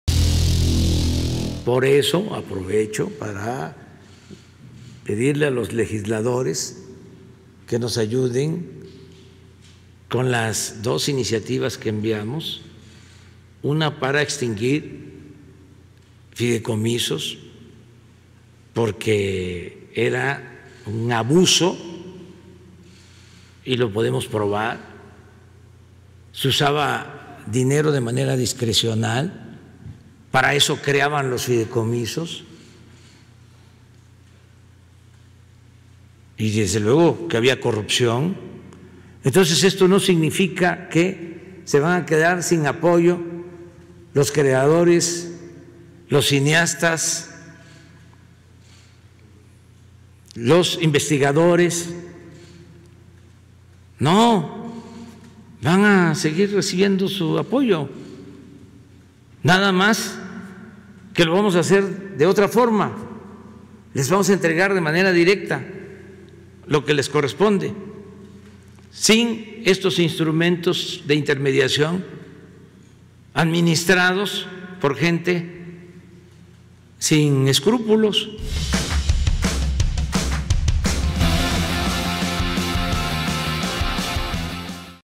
AMLO busca desaparecer el Fodepar y otros fideicomisos
El Fodepar reparte 298 millones de pesos de más de 700 beneficiarios